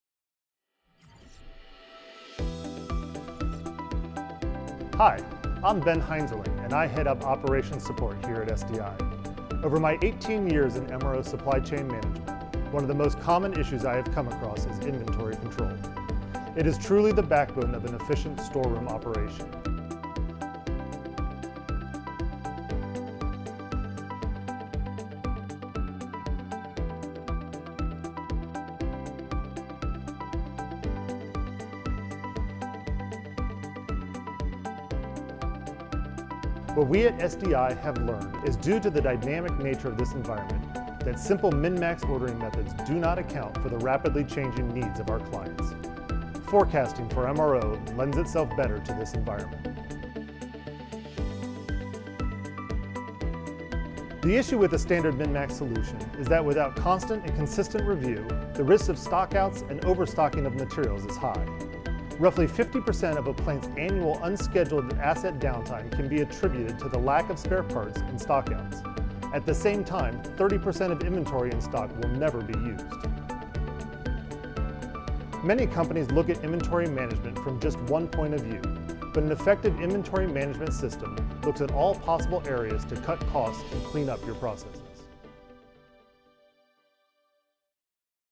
Facility Maintenance Parts & Supplies Expertise on Inventory Optimization.
SDI's Benjamin Heinzerling shares insights on why traditional min/max ordering methods are not effective in today's dynamic environment.

Consider that 25% of the annual budget for Facility Maintenance is spent on parts and supplies AND that 30% of the inventory in stock will never be used. Yet half of all unscheduled downtime and facility disruptions are caused because of stock-outs.

It's time to Think Differently About Inventory Control!